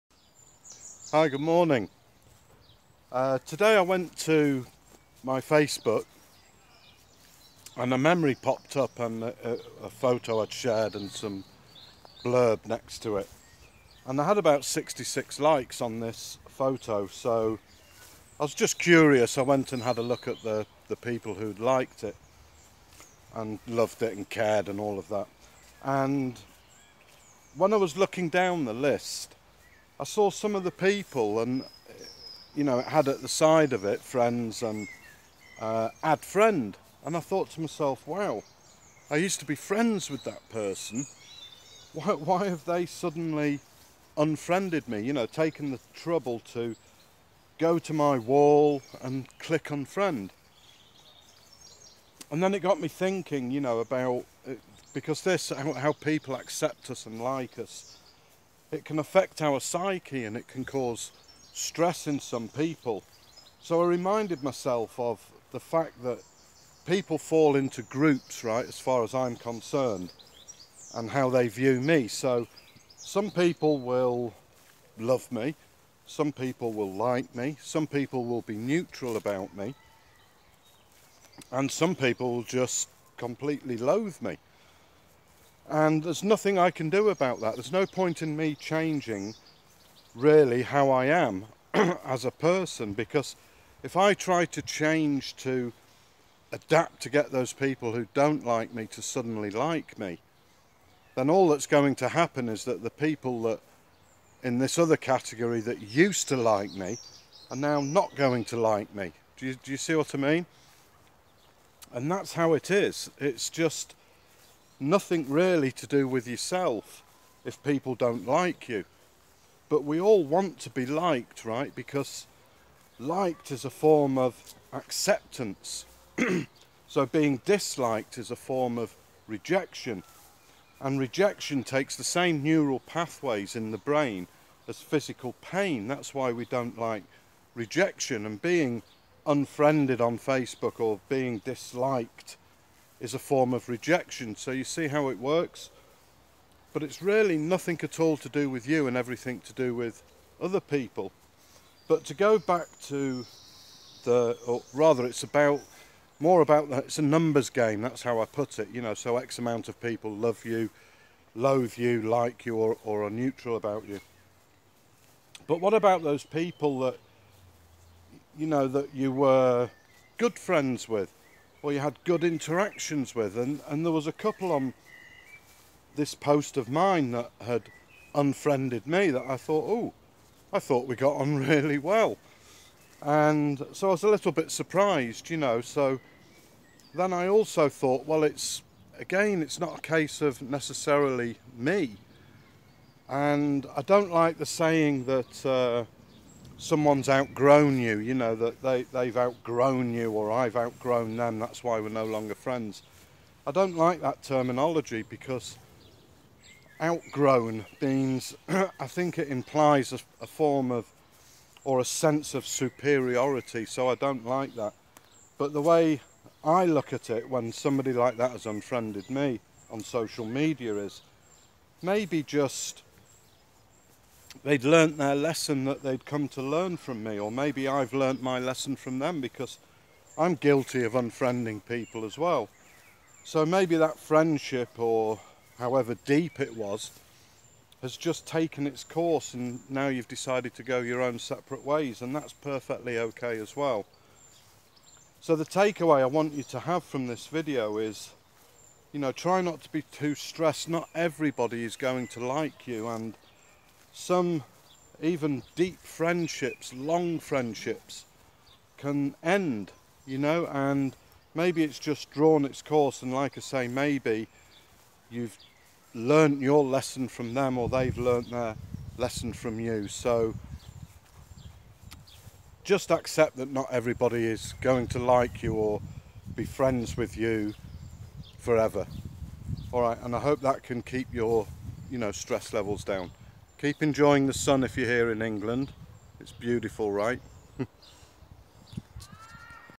Why People Unfriend or Don't Like You
We can get hurt when people don't like us and especially when friends unfriend us on social media.

In this video, I explain why it hurts not to be liked or when a contact unfriends you on social media.
I also explain why it's not your fault and why trying to change won't win anyone back.